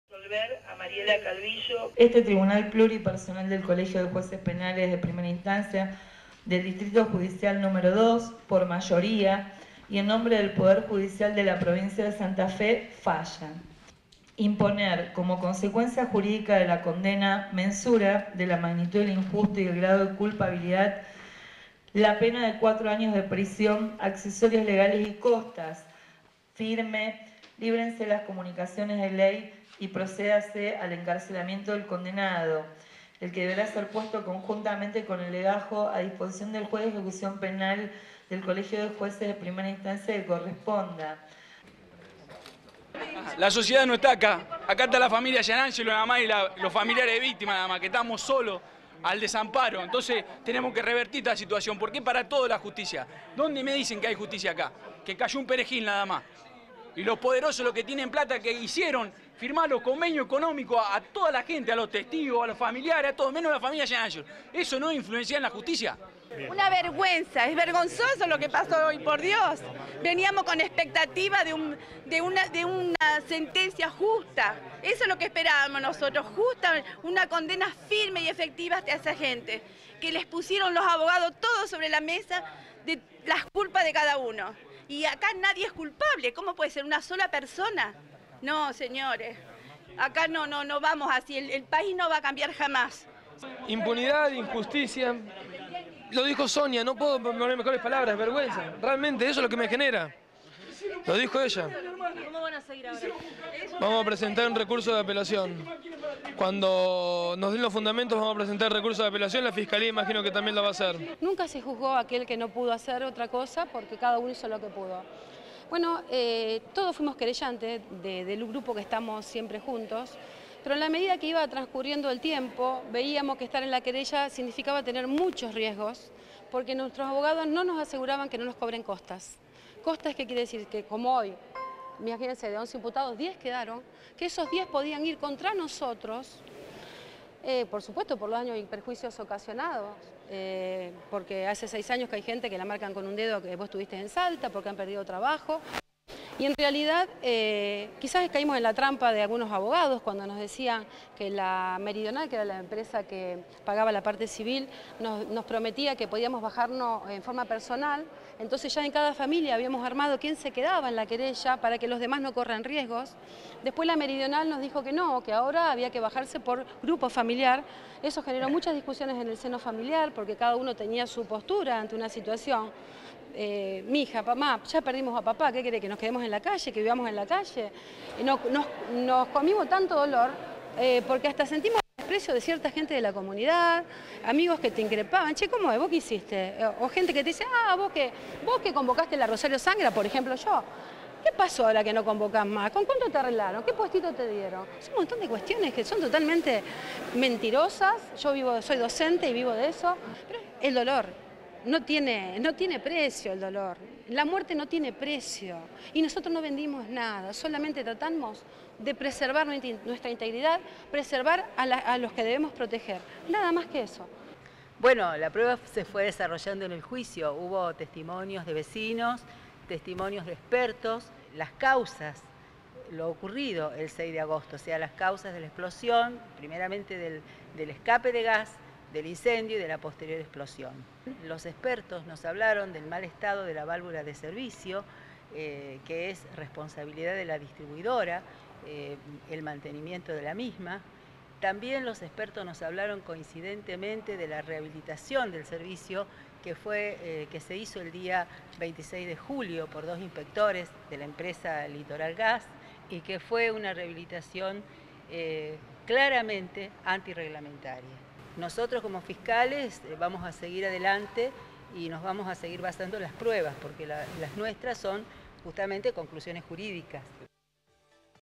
Salta2141. Indignación de familiares por el fallo. Apelarán la resolución del tribunal.
APELARÁN LA RESOLUCIÓN DEL TRIBUNAL
ADRIÁN GIANÁNGELO – FAMILIAR DE VÍCTIMA
SONIA GIANÁNGELO – FAMILIAR DE VÍCTIMA
ANGELO ROSINI – ABOGADO DE  LA FAMILIA GIANÁNGELO
TOTAL DESACUERDO CON EL FALLO ABSOLUTORIO DEL TRIBUNAL
MARCELA NISORIA – FAMILIAR DE VÍCTIMA

EL MINISTERIO PÚBLICO AGOTARÁ TODAS LAS INSTANCIAS
GRACIELA ARGÜELLES – FISCAL
Condenaron al gasista Carlos García a cuatro años de prisión y el resto de
los acusados fueron absueltos. De los once acusados, sólo se le atribuyó
responsabilidad penal al gasista que el 6 de agosto de 2013 se encontraba
en el edificio de Salta 2141. Hubo fuertes protestas de los familiares.